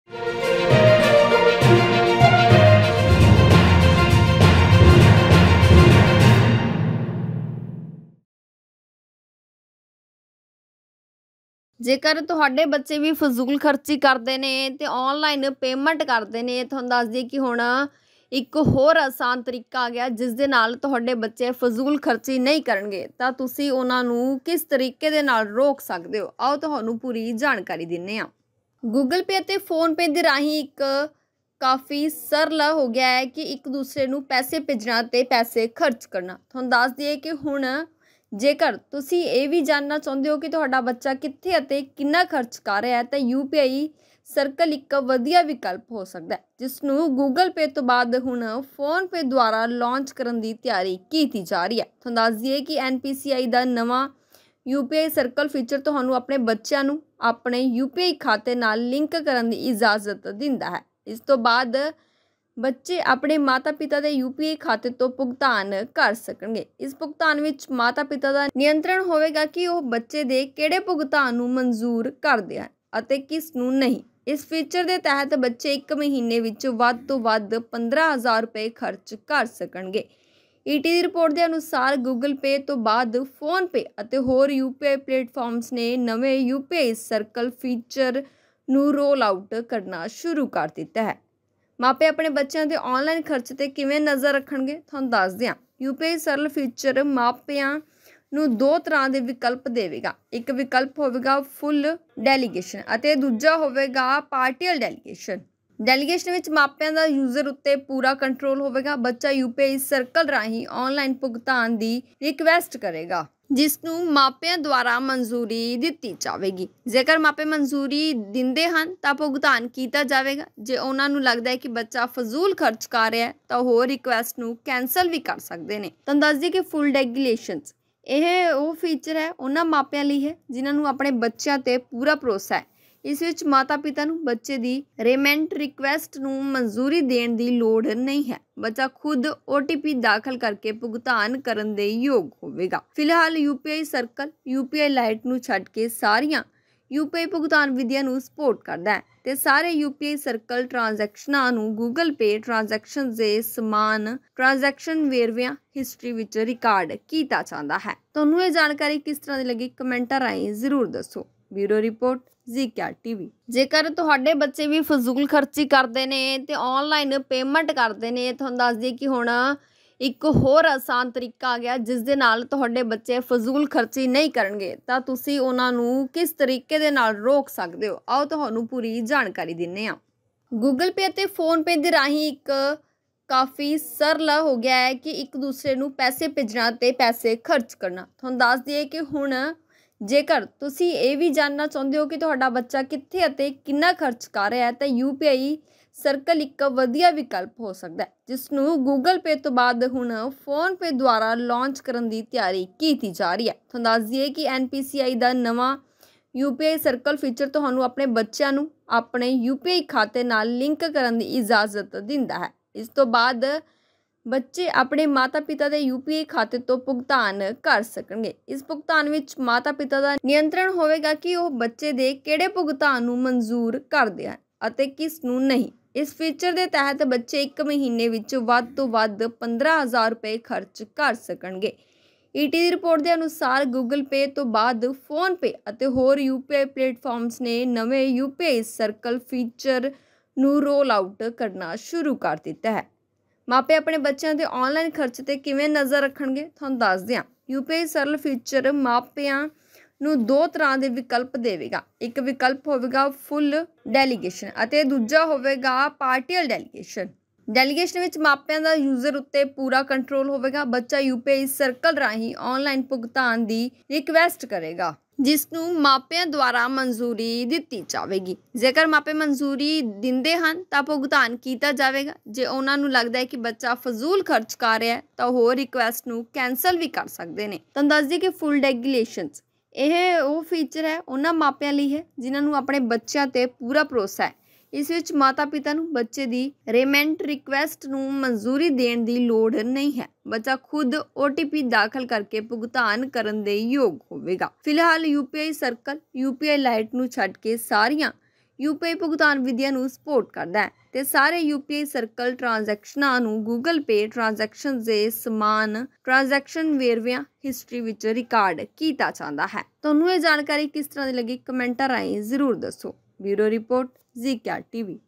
ਹੁਣ ਬੱਚੇ ਬੇਫਿਜੂਲ ਆਨਲਾਈਨ ਪੇਮੈਂਟ ਨਹੀਂ ਕਰ ਸਕਣਗੇ... ਹਰ ਪੇਮੈਂਟ 'ਤੇ ਰਹੇਗੀ ਮਾਪਿਆਂ ਦੀ ਨਜ਼ਰ!
GKR TV
GKR NEWS